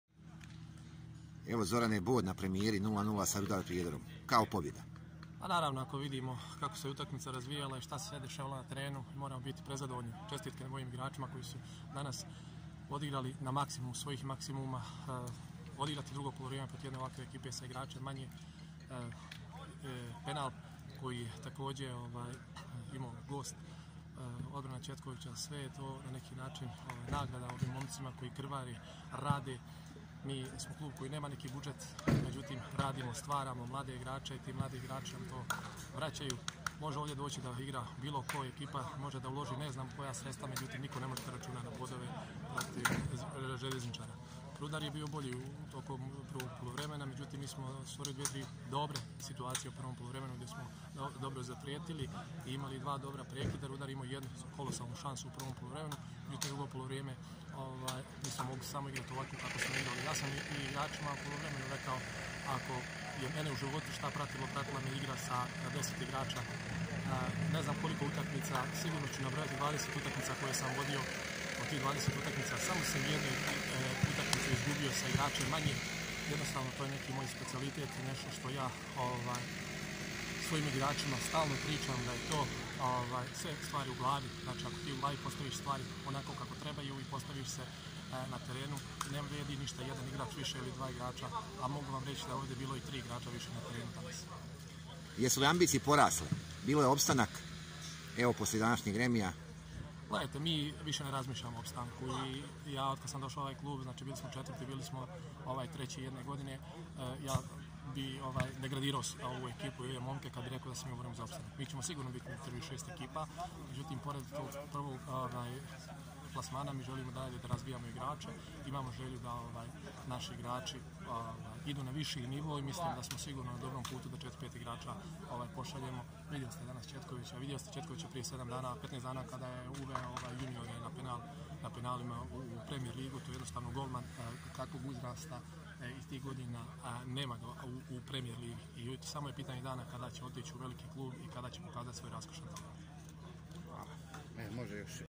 Zoran Ćurguz, trener FK Željezničar Sport team
Remi na "Predgrađu" između Želje i Rudar Prijedora. Zadovoljniji strateg domaćina...